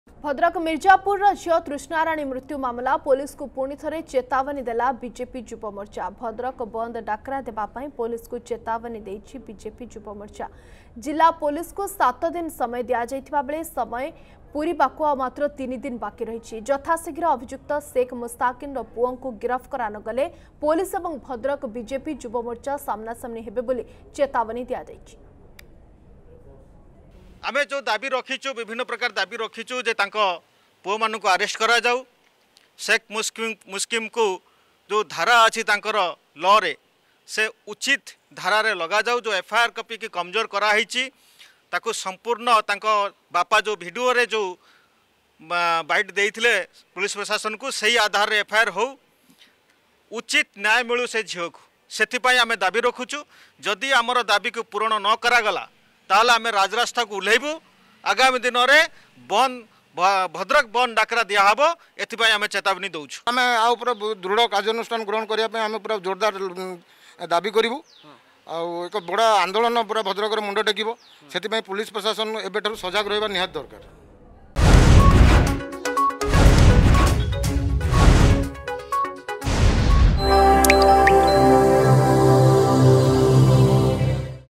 ଭଦ୍ରକ ମିର୍ଜାପୁରର ଝିଅ ତୃଷ୍ଣାରାଣୀ ମୃତ୍ୟୁ ମାମଲା
ଭଦ୍ରକ ମିର୍ଜାପୁରର ଝିଅ ତୃଷ୍ଣାରାଣୀ ମୃତ୍ୟୁ ମାମଲା । ପୋଲିସକୁ ପୁଣି ଥରେ ଚେତାବନୀ ଦେଲା ବଜେପି ଯୁବ ମୋର୍ଚ୍ଚା ।  ଭଦ୍ରକ ବନ୍ଦ ଡାକରା ଦେବା ପାଇଁ ପୋଲିସକୁ ଚେତାବନୀ ଦେଇଛି ବିଜେପି ଯୁବ ମୋର୍ଚ୍ଚା